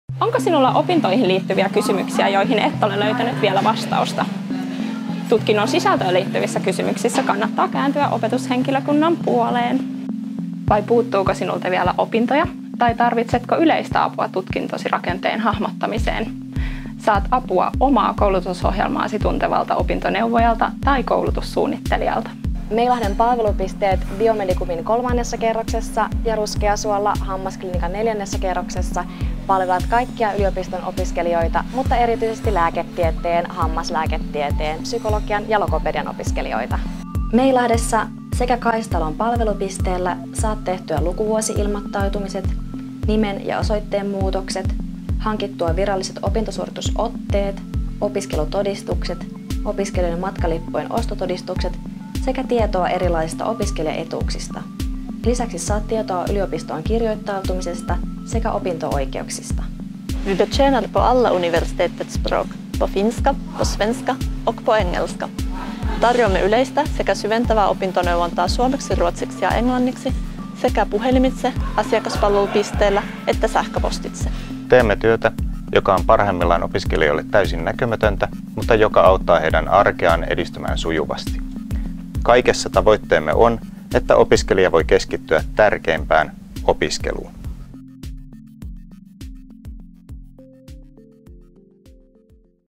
Meilahden opiskelijapalvelut opiskelijan tukena
Meilahden opiskelijapalvelut tukee lääketieteellisen tiedekunnan opiskelijoita kaikissa heidän opintoihinsa liittyvissä asioissa, sekä näkyvästi neuvonnassa että näkymättömästi suunnittelussa. Hammaslääketieteen, logopedian, lääketieteen, psykologian ja translationaalisen lääketieteen opiskelijat saavat tutkintoihin ja opintoihin liittyvää neuvontaa.